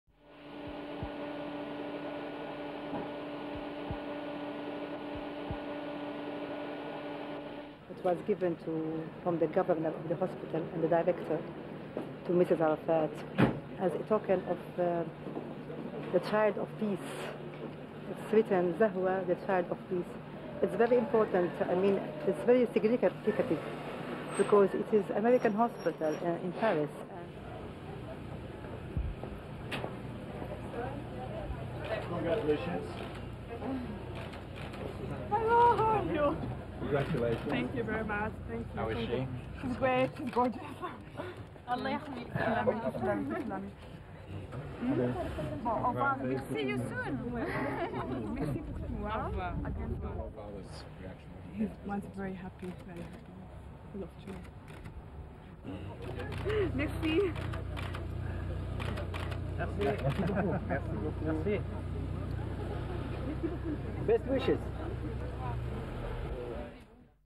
France - For Yasser It's A Girl
(14 Aug 1995) T/I 10:23:14

FOR YASSER IT'S A GIRL

Paris, France            Natsot                   Duration: 1.07"

Suha Arafat could not conceal her joy at new family member Zahwa as she left the American Hospital in Paris accompanied by her mother Raimonda Tawil. Zahwa is the first child of 65 year-old PLO leader Yasser Arafat and his 33 year-old wife Suha. Zahwa, who weighed in at 2.4kg when she was born at the American Hospital in the Paris suburb of Neuilly, is apparently very like her father. Arafat saw his daughter on the day after she was born on 25 July. Before her birth Suha Arafat had said she wanted a little girl, while her husband preferred a son. The PLO chief celebrated his 65th birthday on the same day his newborn daughter went home from the hospital.

SHOWS:

Exterior of American Hospital in Paris. Hospital entrance. Cup given by hospital and staff. SOT Raimonda Tawil (Yasser Arafat's mother-in-law): "It was given from the government of the hospital to Mrs Arafat as a token of the sign of peace. It's written "Zahwa, child of peace."... "It's very important. It's very significant because it is American Hospital in Paris." Mrs Suha Arafat leaving hospital with baby. SOT Mrs  Arafat: "Thank you very much. She's great, she's gorgeous." CU mother and baby. Mrs Arafat and baby. SOT doctor: "See you soon." SOT Mrs Arafat in car: "Merci." Car driving away.